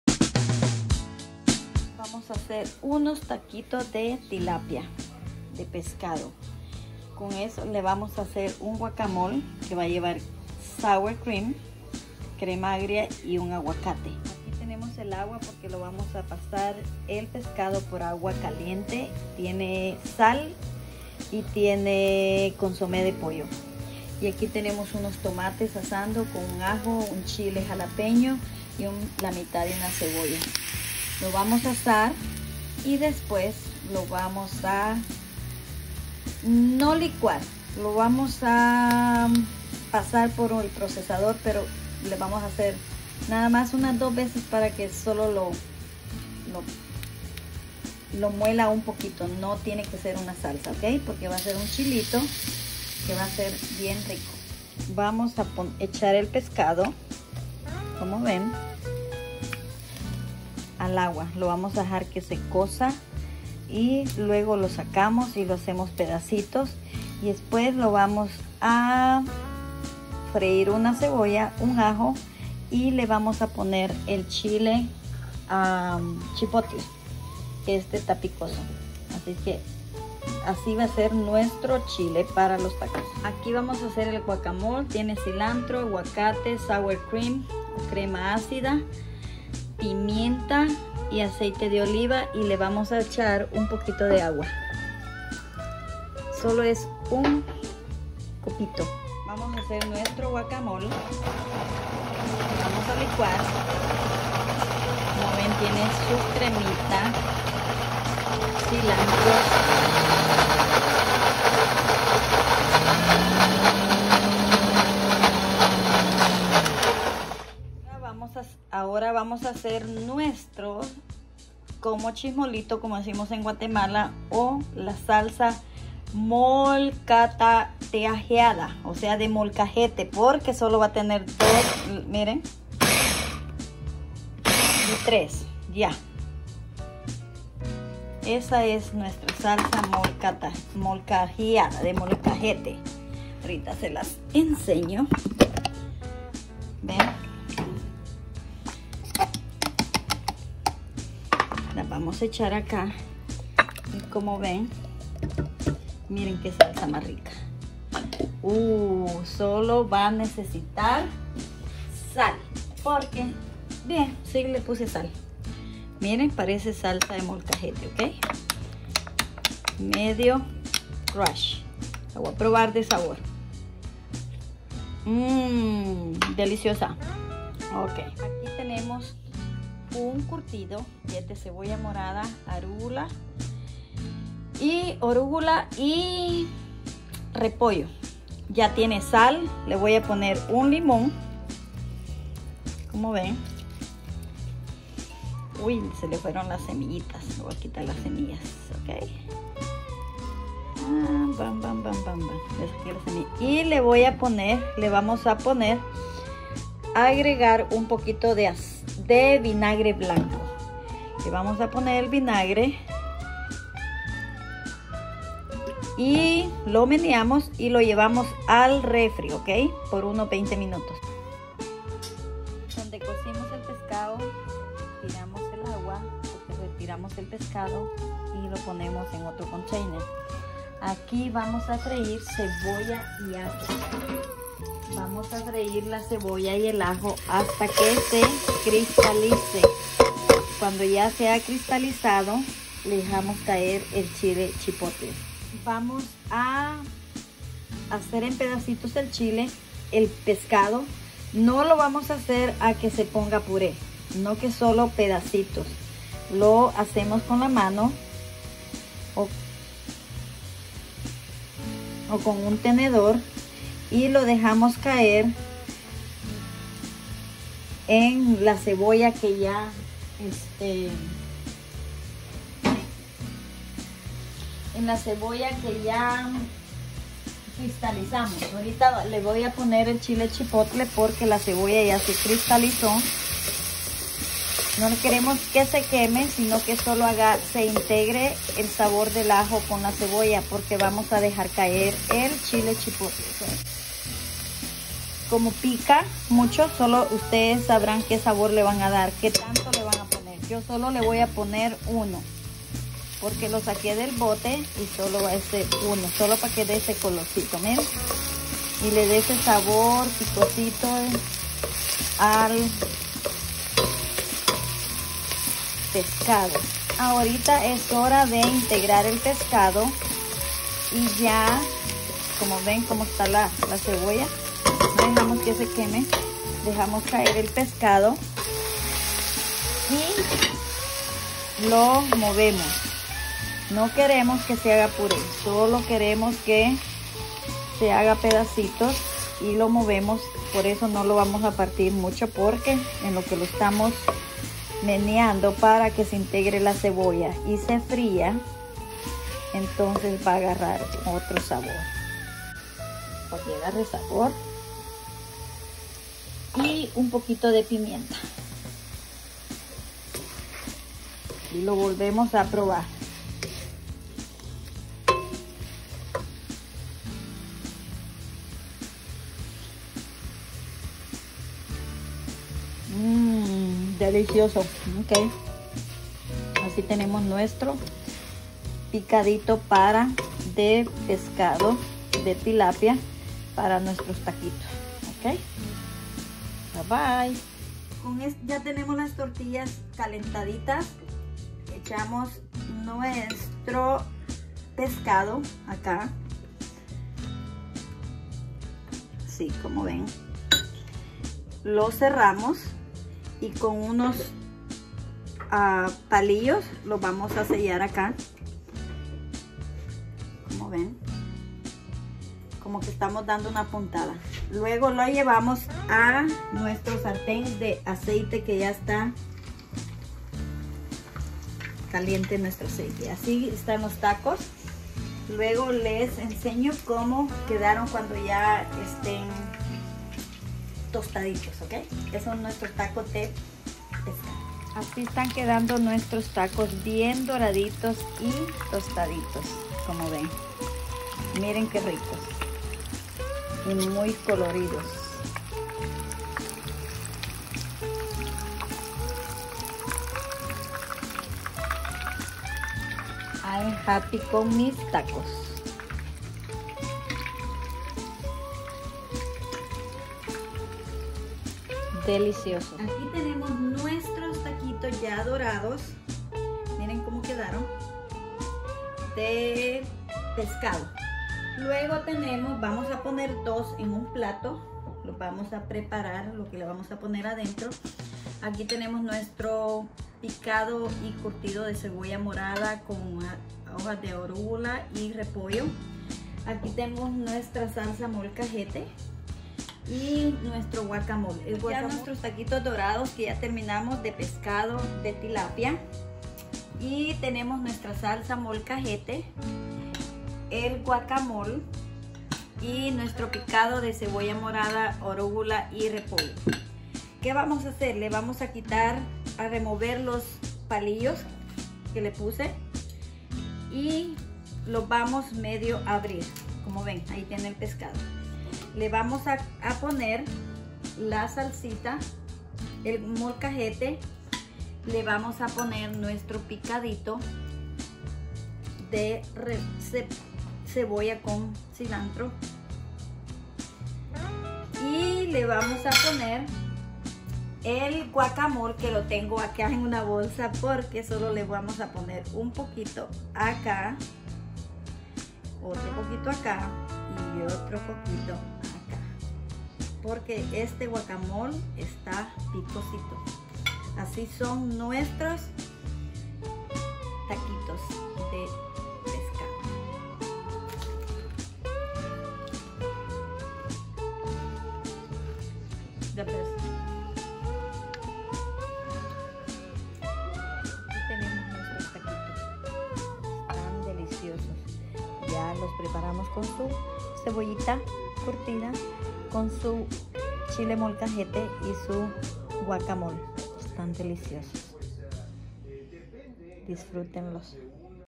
Taquitos Dorados D Tilapia
Pescado tilapia, condimentos, tortillas d maizena , ensalada d repelling, y salsa verde d aguacate